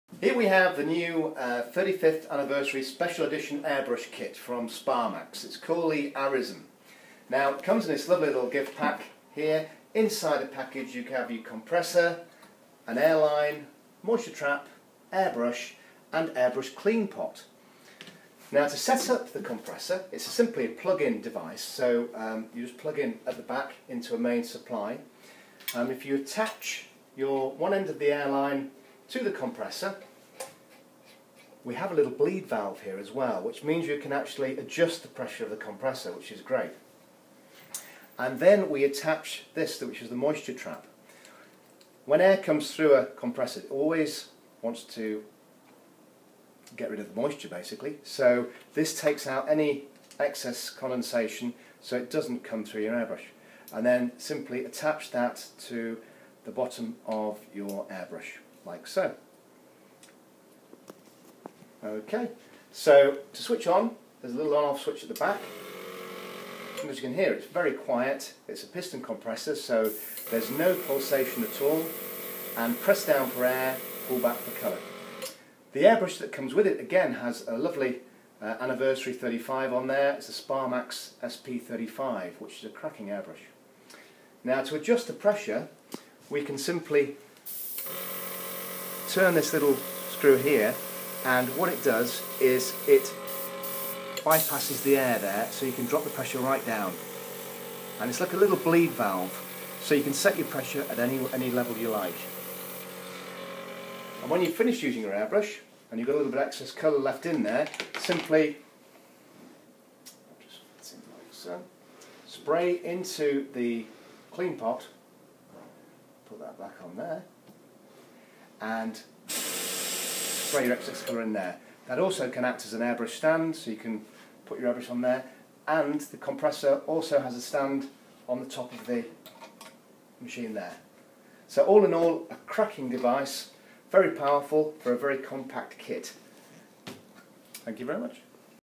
Sparmax Airbrush Compressor kit 35 year anniversary Limited edition
A very well equipped Airbrush Compressor kit made by Sparmax to celebrate their 35th year anniversary. This perfect gift contains all your need and more for all kinds of airbushing including general artwork, Airbrush Make-up, Tanning, Naile art and much more.
graphicsdirect.co.uk